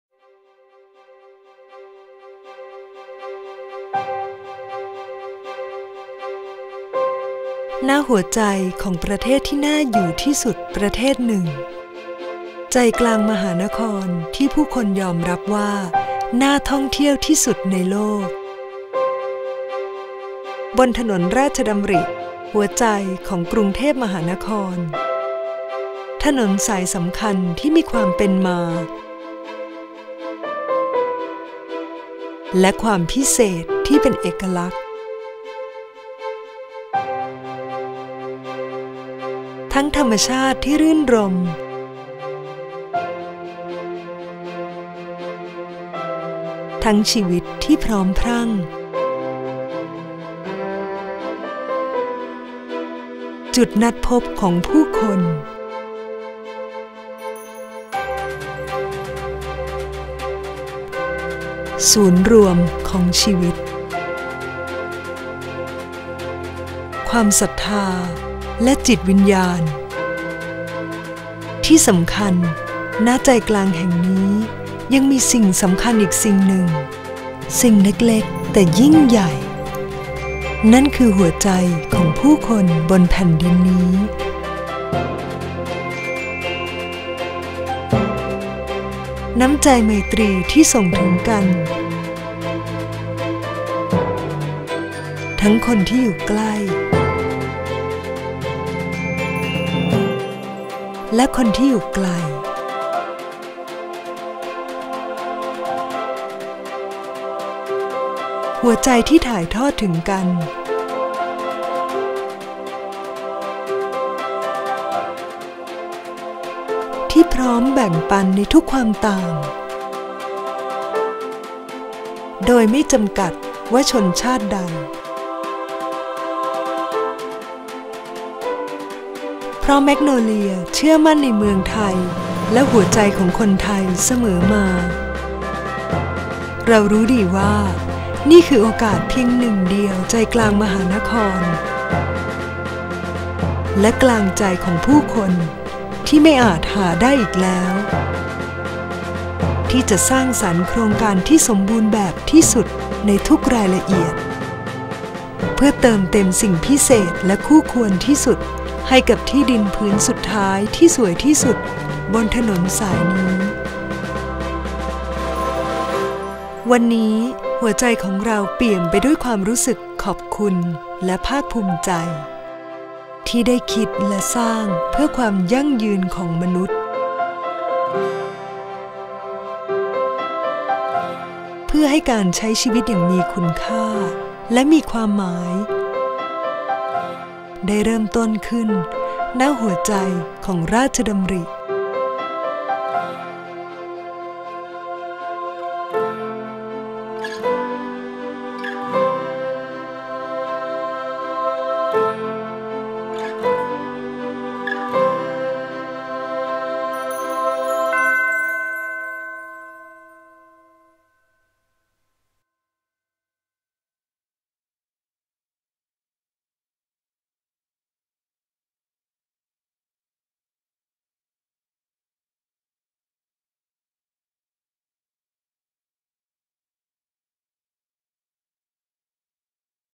Magnolias Ratchadamri Boulevard : Fulfilling The Legacy
At the heart of a country held in the highest regard as a place to live, at the heart of a dream city for international travellers, there is a prestigious boulevard that is a crossroads of the city's life and spirit. Presenting a uniquely superior world of fine architecture and green vistas, of outstanding hotels and entrancing diversions, Ratchadamri Boulevard offers the perfect home.

At the heart of this place lies something supremely important... the heart of the people of this great land. Amid warm Thai smiles and loving kindness is an exceptional opportunity that cannot be found elsewhere, an ideal home on the last available and finest site on this incomparable avenue.

Enjoy a life with more meaning and greater value. Here at the heart of Ratchadamri. At Magnolias Ratchadamri Boulevard.